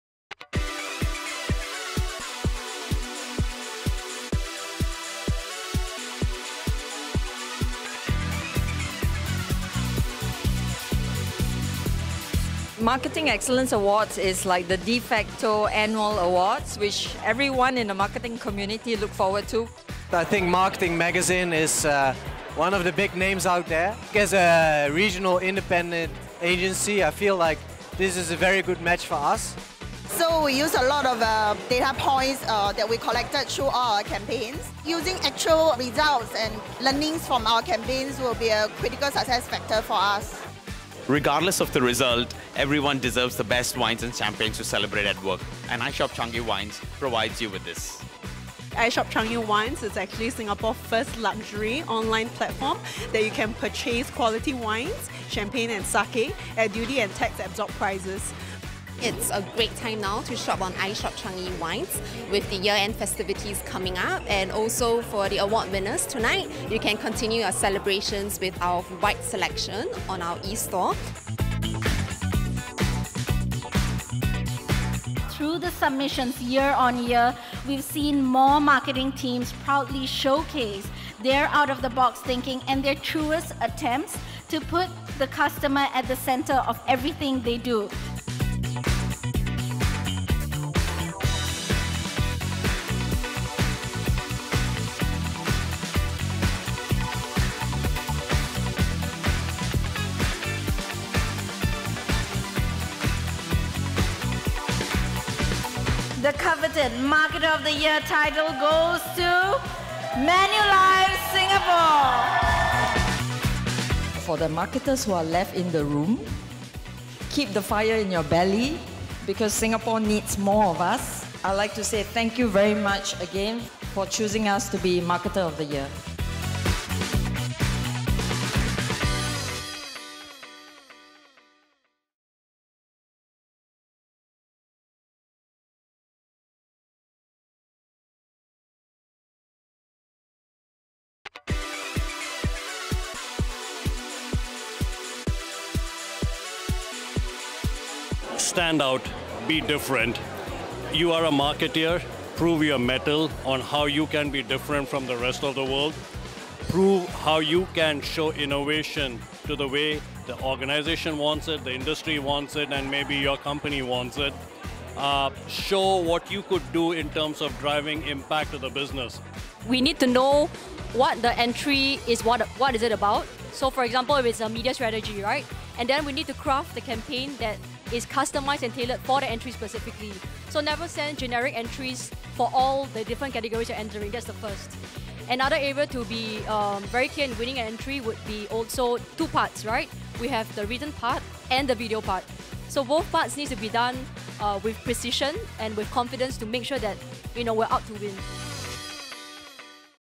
Marketing Excellence Awards 2019 Singapore - Video Highlights
Watch our Marketing Excellence Awards 2019 Singapore Highlights!

Manulife Singapore claimed the coveted “Marketer of the Year” crown at the Marketing Excellence Awards 2019 having earned seven gold, two silver and four bronze trophies. Coming in second place was DBS Bank which walked away with two gold, five silver and four bronze trophies for seven of its campaigns.

Congratulations to all of our finalists and outstanding winners – we look forward to seeing more of your excellence in 2020!